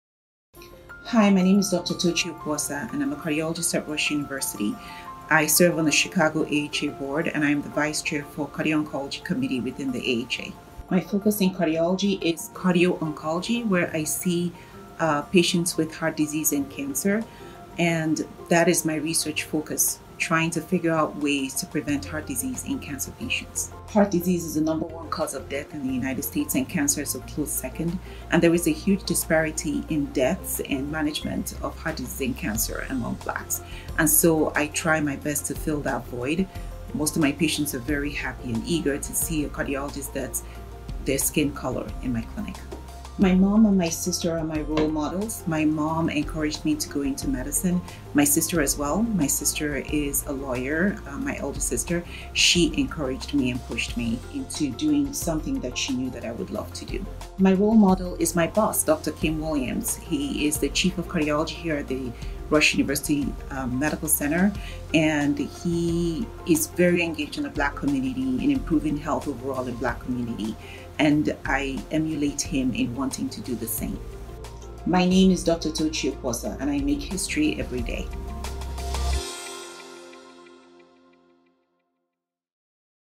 Tochukwu Okwuosa- Black Americans Making History Every Day
Researchers. Entrepreneurs. STEM students. Community advocates. These young Americans are making Black history every day across the USA. Celebrate their stories. Then make yours.